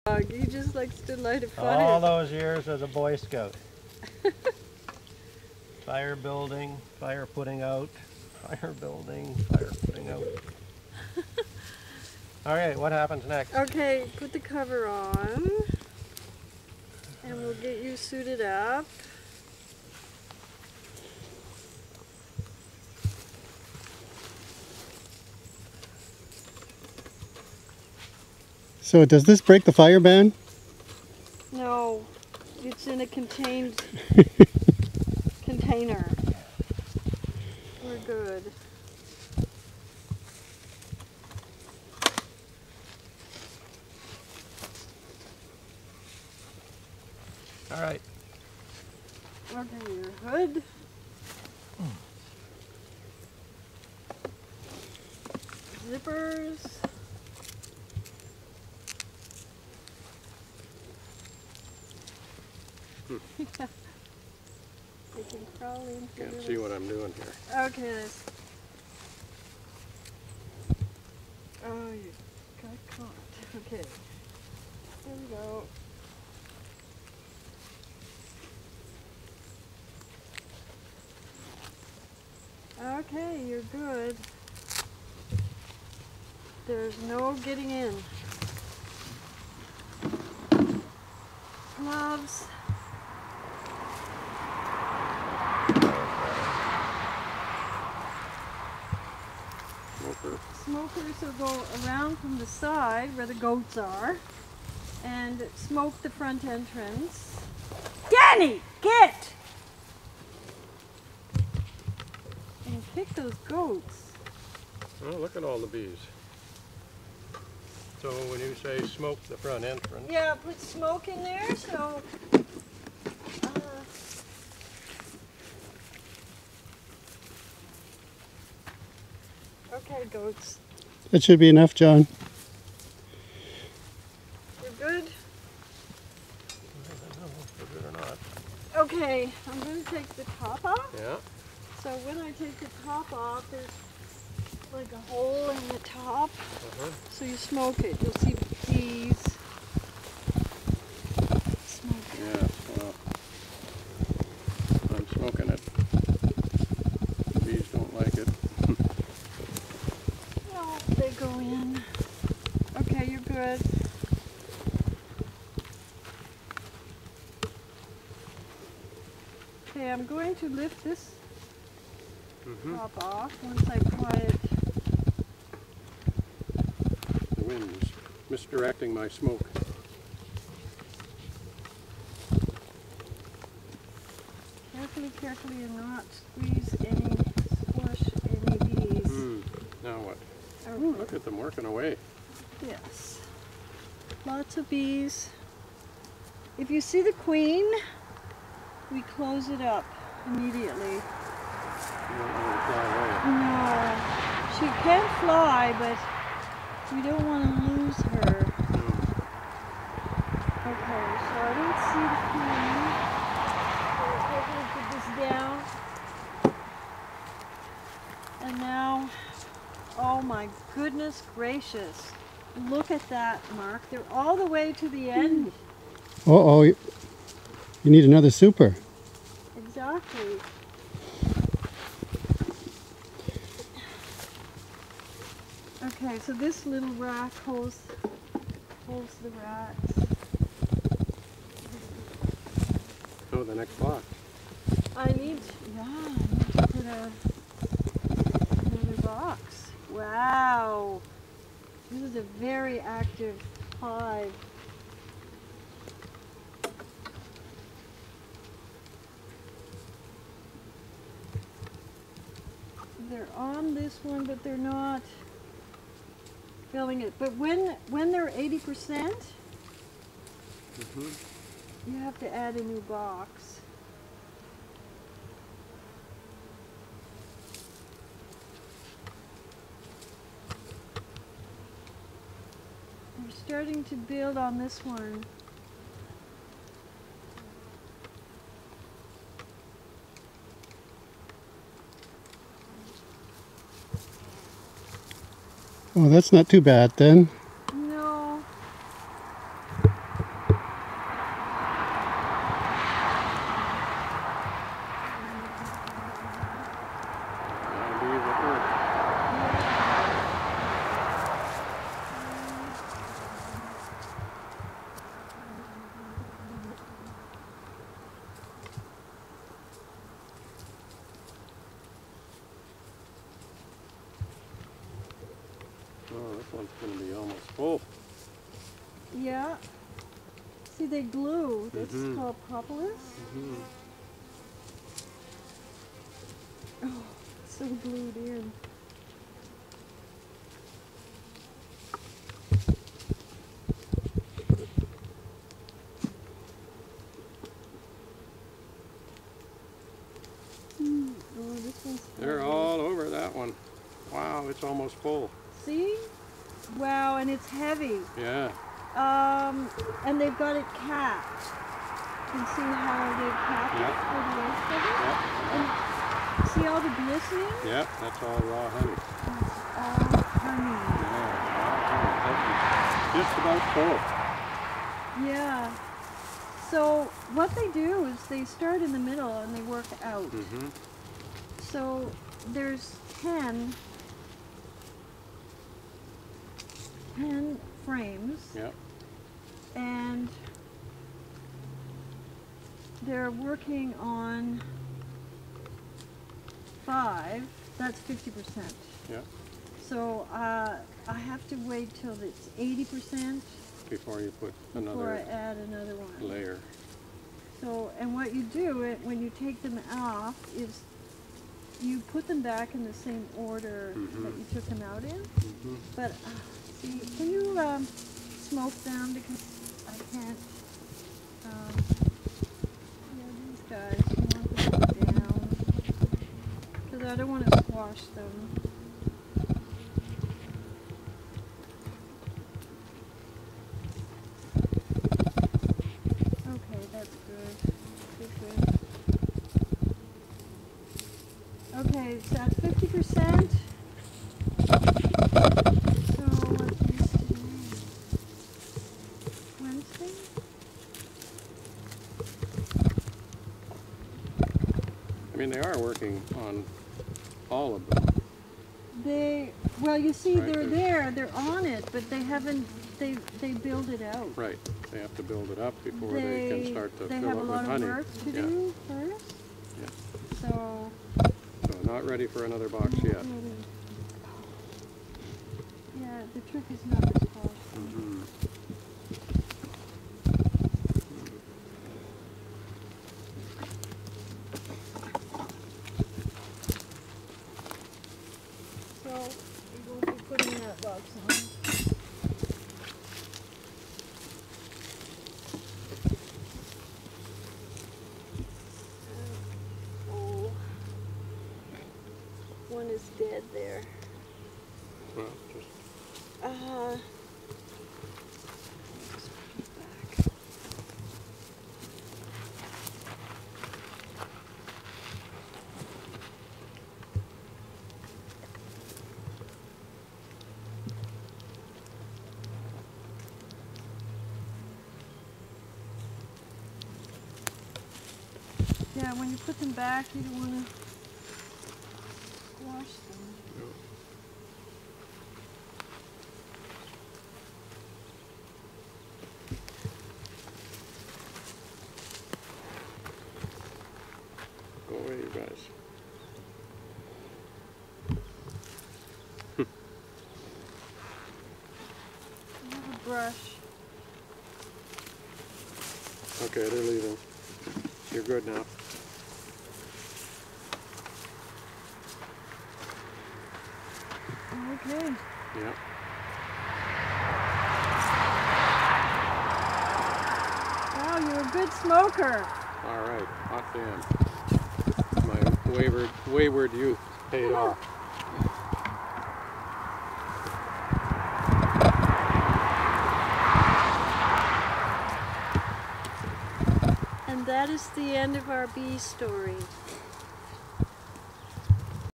Checking the Beehive
Time to check on the health of our beehive. John and the goats are helping.